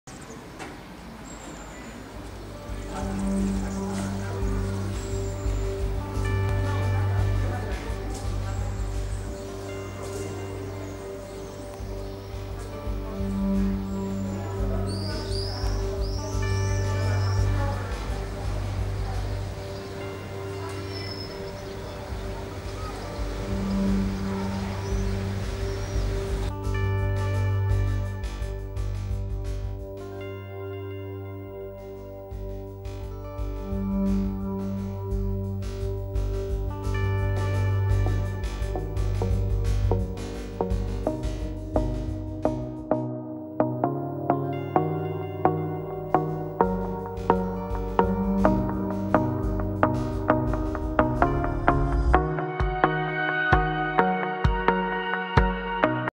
PRESENTACIÓ projecte art CONNEXIONS CARDENER
Inaugurem l'exposició dels FANZINES i el web del projecte d'art CONNEXIONS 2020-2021